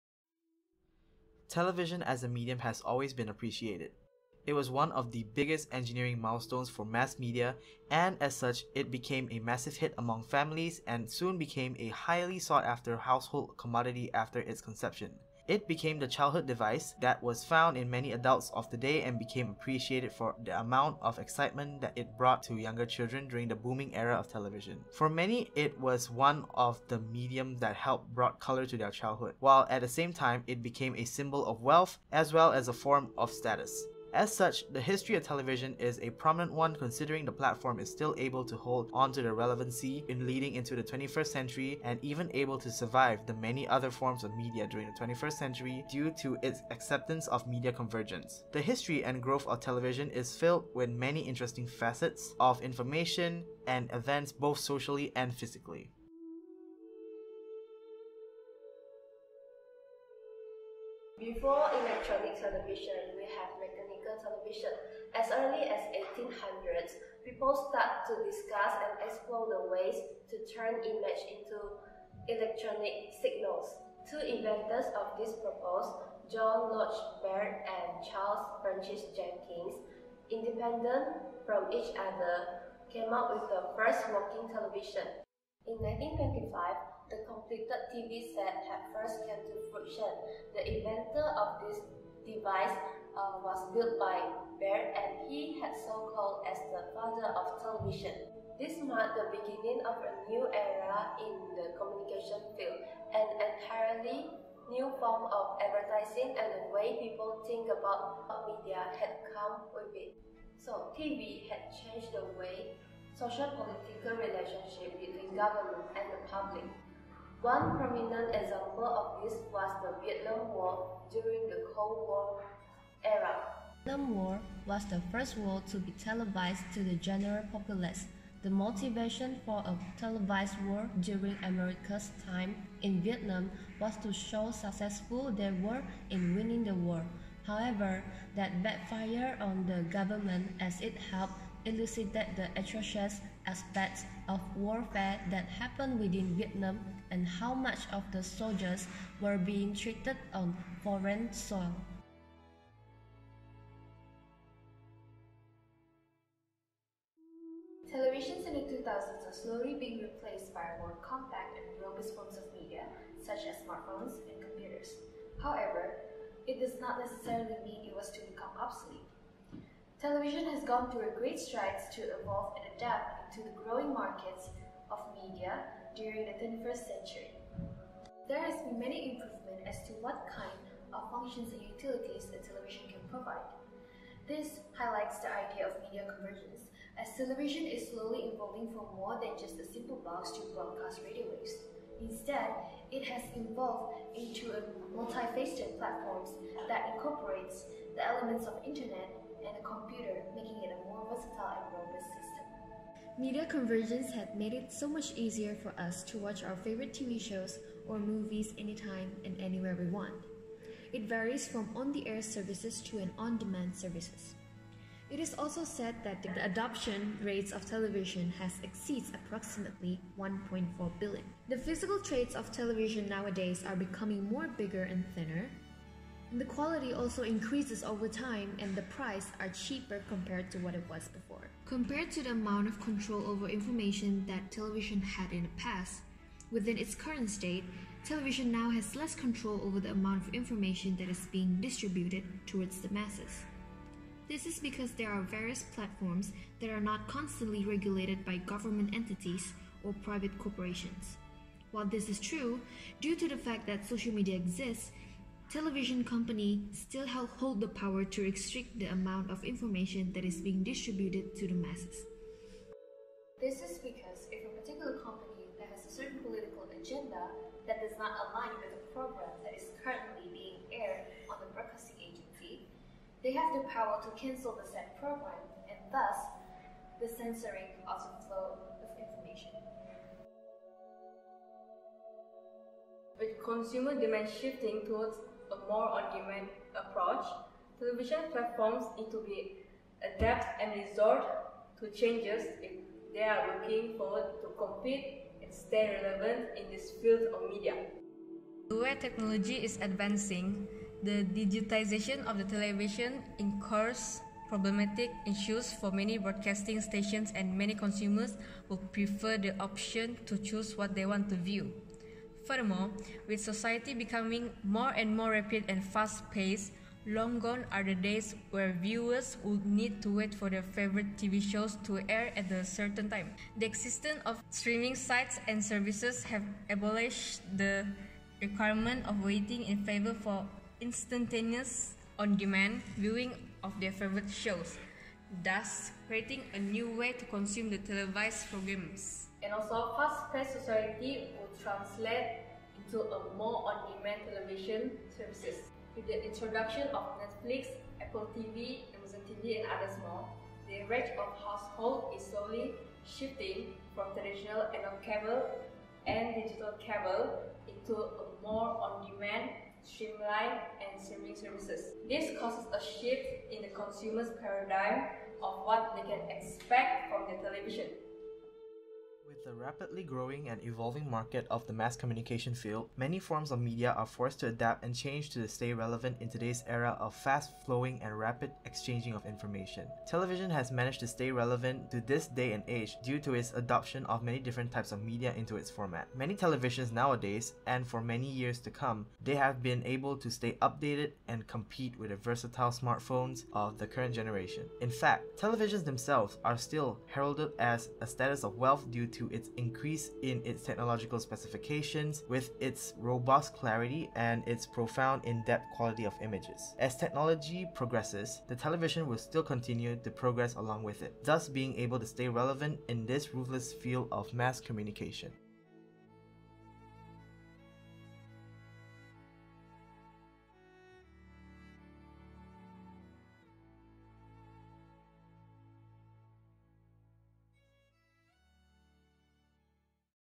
The Evolution of Television | Mini Documentary
This was an assignment for Introduction To Mass Communication subject for Diploma in Mass Communication students in SEGi College Sarawak.